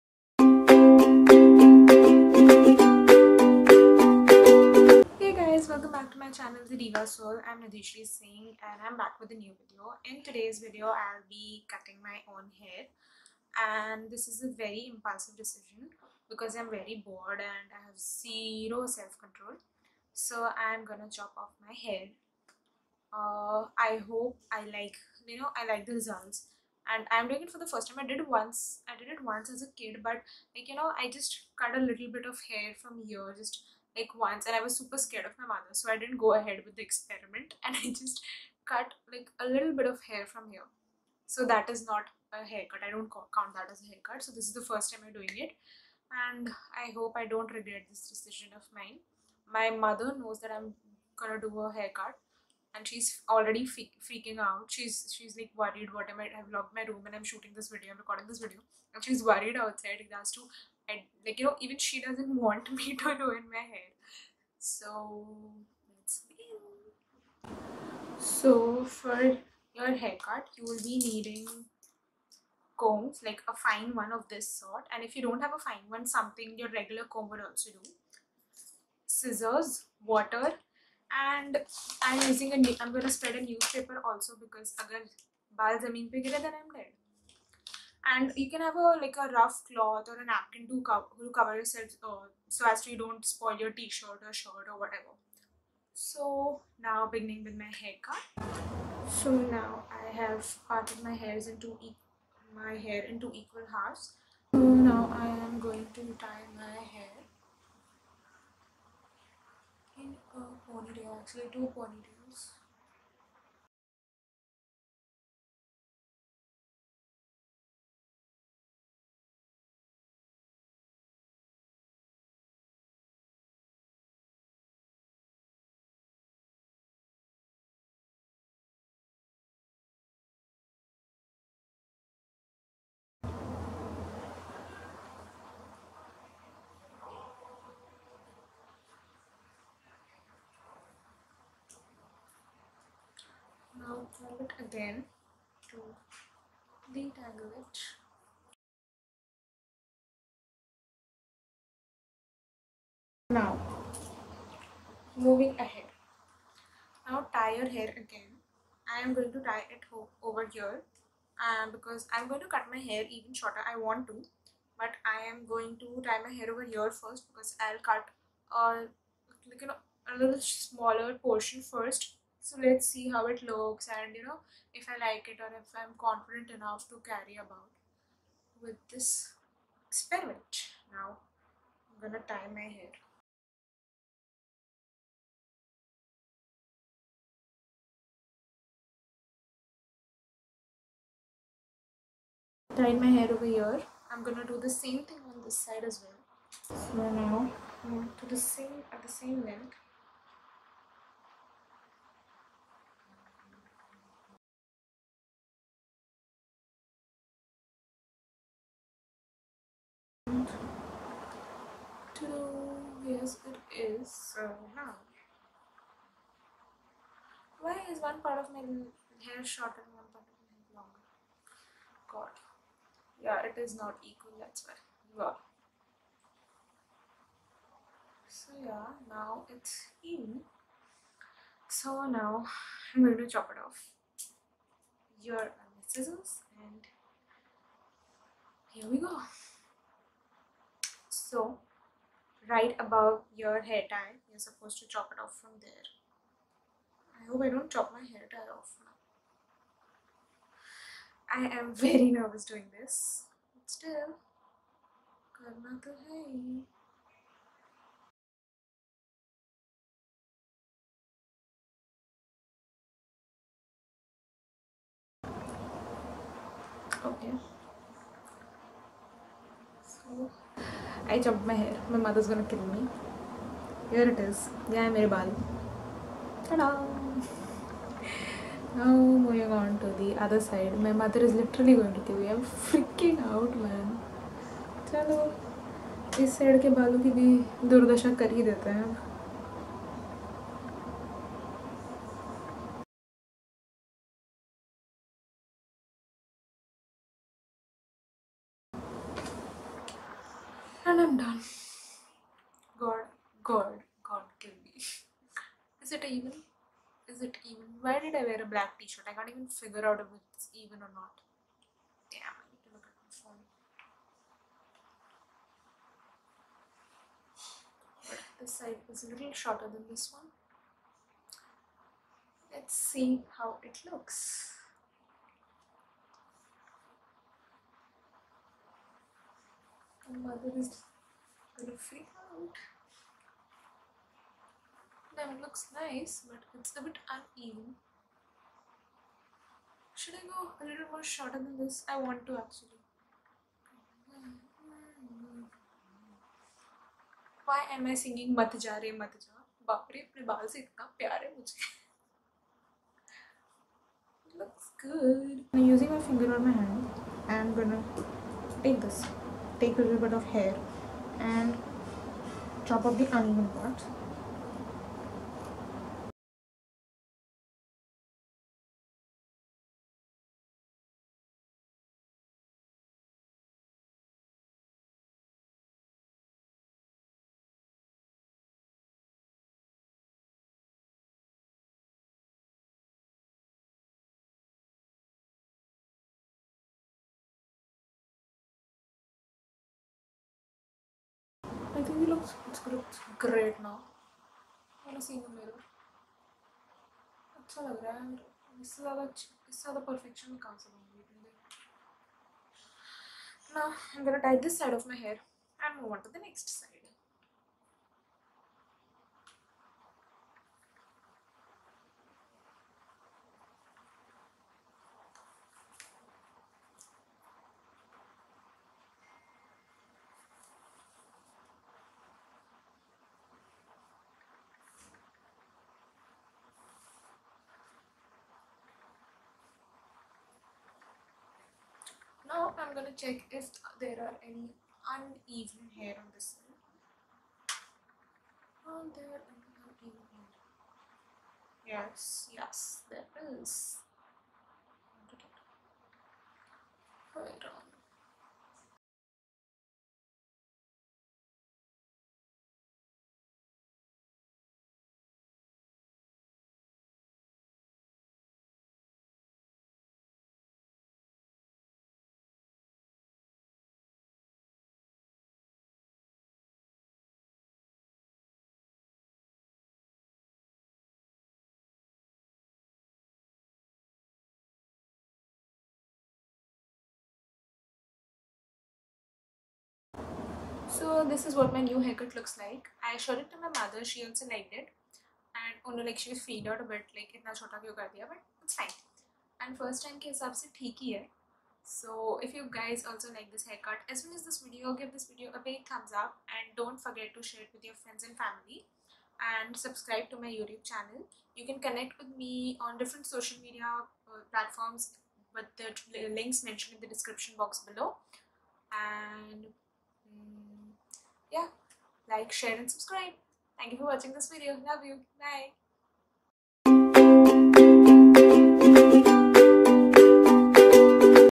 How I cut my own hair for the first time | Chatty video | Nidhishree Singh
In this chatty video, watch how I cut my hair at home because I am bored during this lockdown and also, see the results of my impulsive decision.